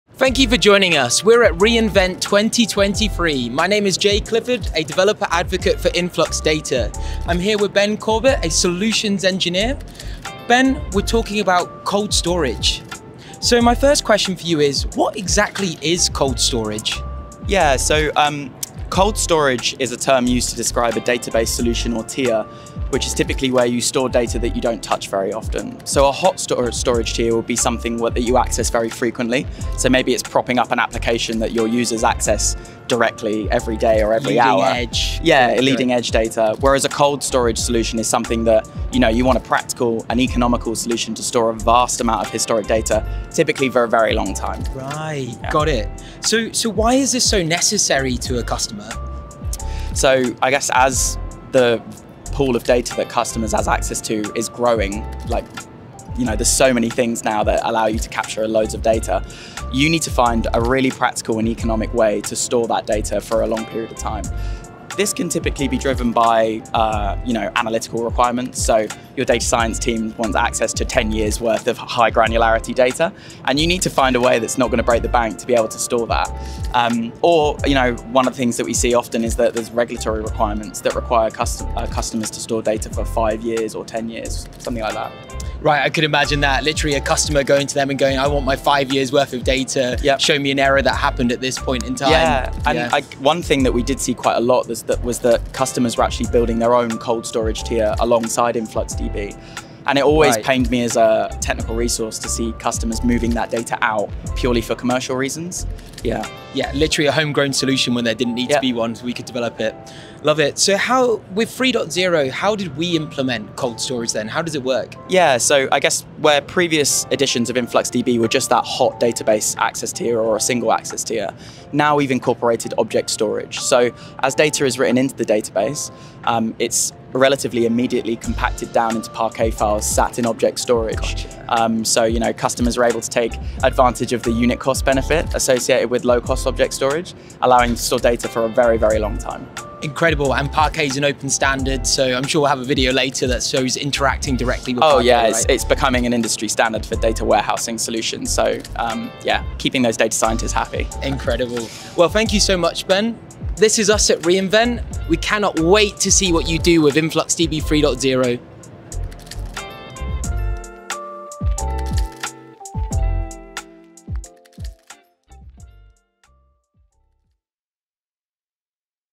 Why Cold Storage Matters for Time Series and InfluxDB 3
More and more, companies rely on high-fidelity, historical data for analytics and to fuel machine learning and artificial intelligence models. Here, Influxers Jay Clifford and Ben Corbett chat about InfluxDB's approach to cold storage and how it helps companies save the data they need for the analytics they want, all without breaking the bank.